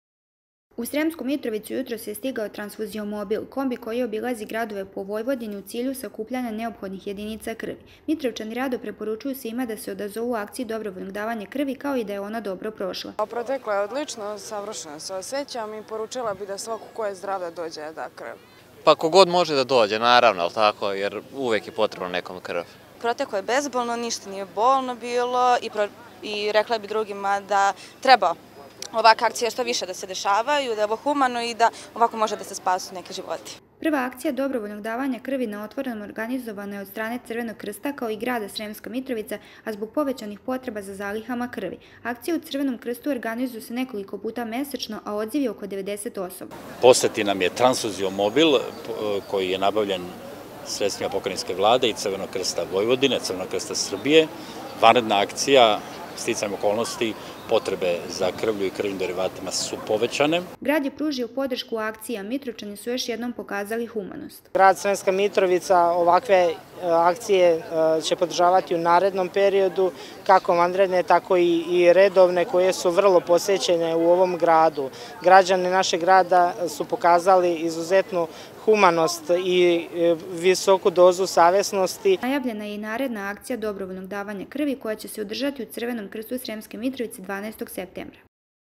Transfuziomobil na Trgu u Mitrovici
Jutros je u Sremsku Mitrovicu na Trg Ćire Milekića stigao transfuziomobil gde će svi građani dobre volje moći da iskažu humanost dobrovoljnim davanjem krvi. Organizatori ove vanredne akcije pored Instituta za Javno zdravlje Vojvodine su Grad Sremska Mitrovica I Crveni krst Sremska Mitrovica.Tokom ovog meseca održaće se još jedna akcija jer je velika potreba za zalihama krvi.